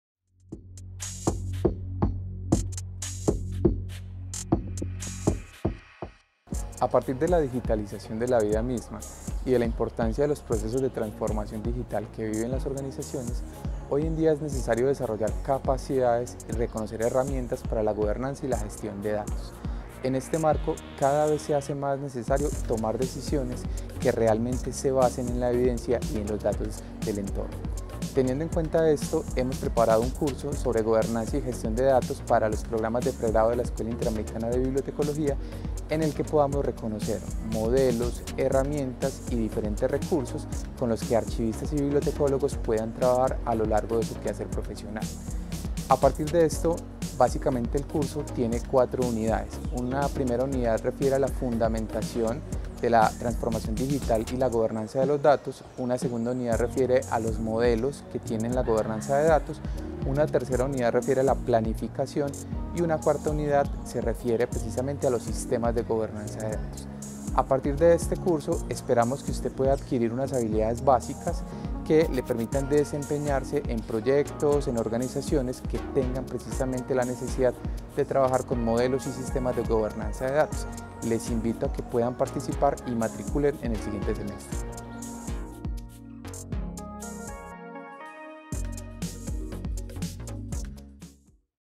Electiva - Gobernanza y gestión de datos
Asignatura electiva de Gobernanza y gestión de datos dictada por el profesor Jaider Ochoa para los pregrados de la Escuela Interamericana de Bibliotecología.